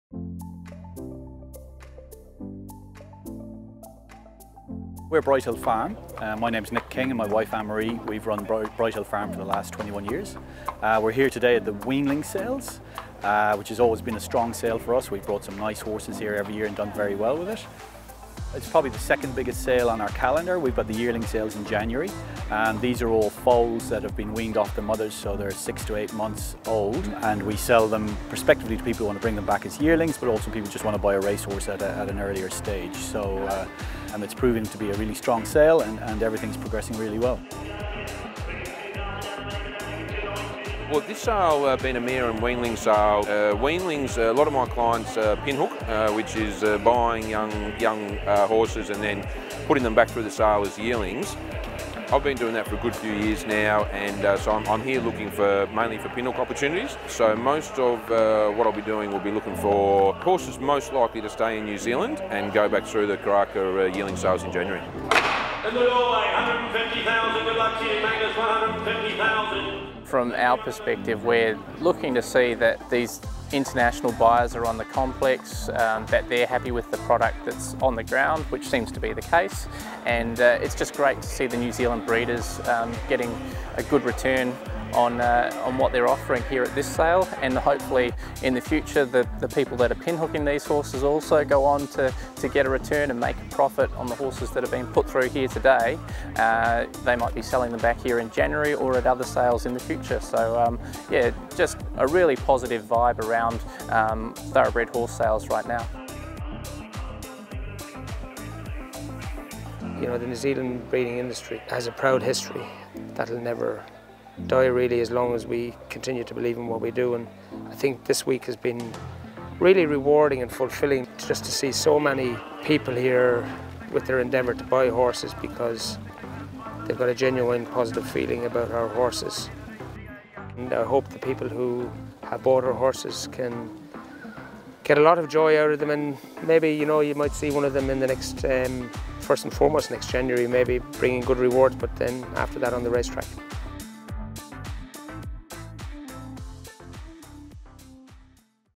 NZ Racing Hall of Fame - Phillip Leishman Media Scholar Film Work - May 2016
Vanessa Vert-Smith (of Country TV) was the 2016 awardee. As part of her work she attended the 2016 National Weanling Sale with a cameraman and interviewed a number of participants.

This short film clip is the result of Vanessa's media work which has been edited into a story by Annie Otway - independent TV producer.